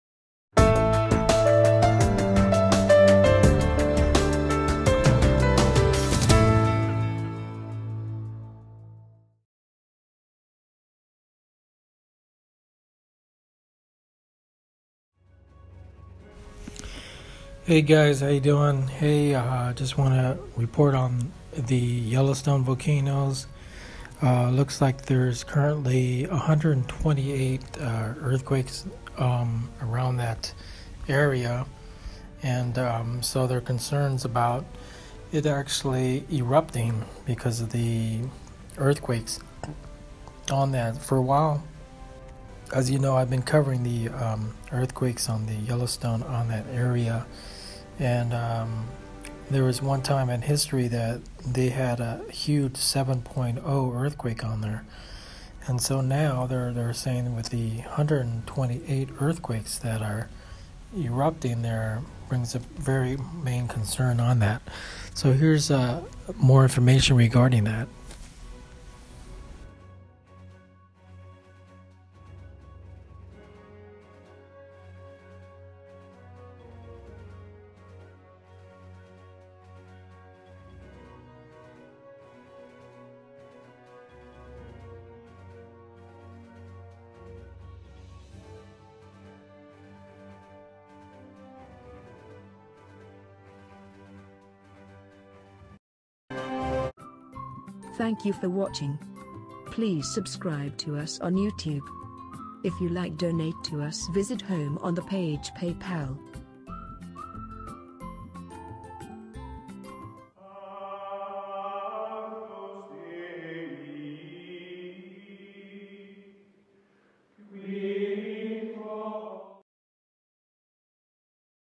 Yellowstone Volcano 128 earthquakes Something Brewing
Yellowstone Volcano 128 earthquakes Something Brewing. Something happening with these earthquakes could it mean that the underneath is brewing